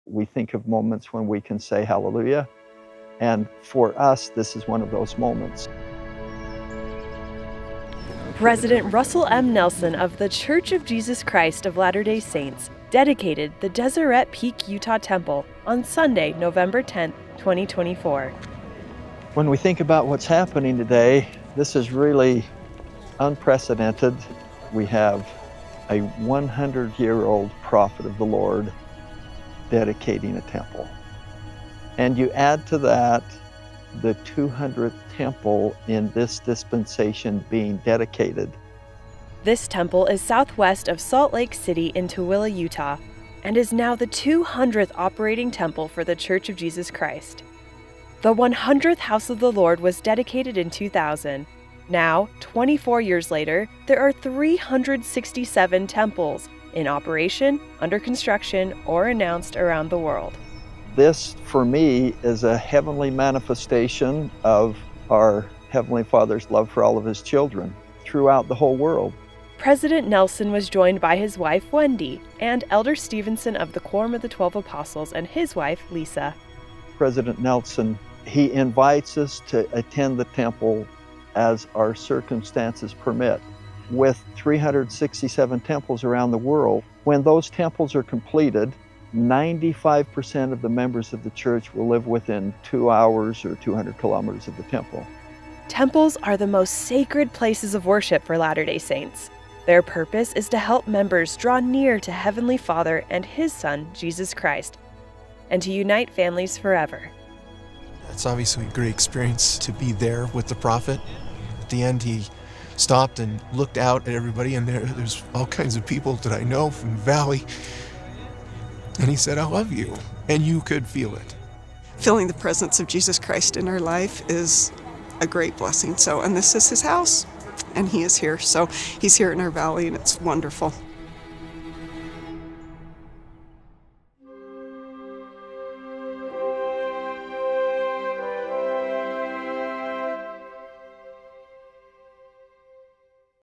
100-Year-Old Prophet Dedicates 200th Temple
President Russell M. Nelson of The Church of Jesus Christ of Latter-day Saints dedicated the Deseret Peak Utah Temple on Sunday, November 10, 2024.

This temple is southwest of Salt Lake City in Tooele, Utah, and is now the 200th operating temple for the Church of Jesus Christ.

The 100th house of the Lord was dedicated in 2000. Now 24 years later, there are 367 temples in operation, under construction or announced around the world.